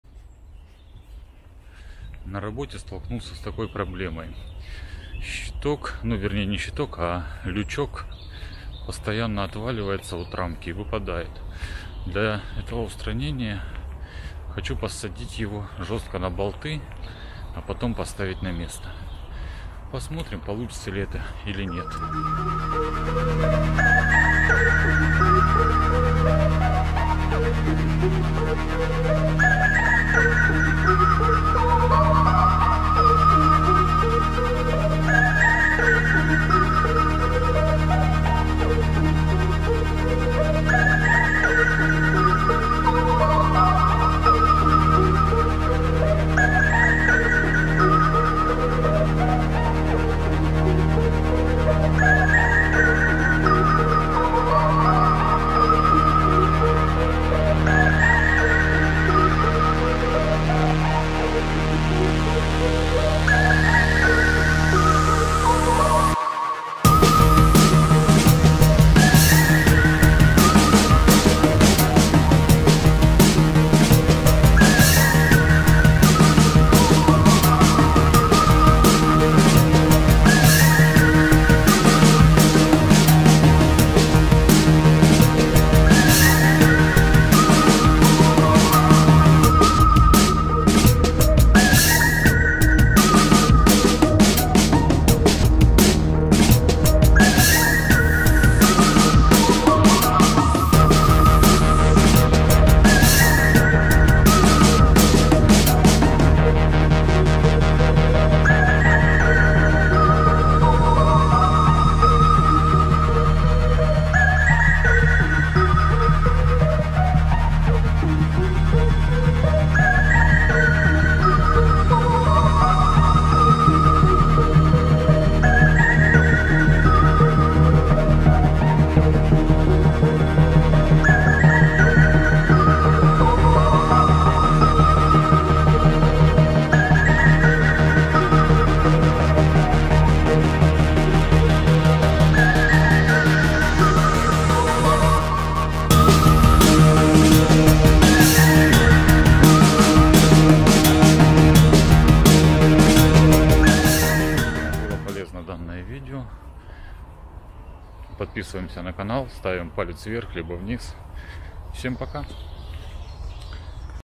Переделываем лючок для ревизии
Переделал лючек ревизии . Теперь он будет не съёмный .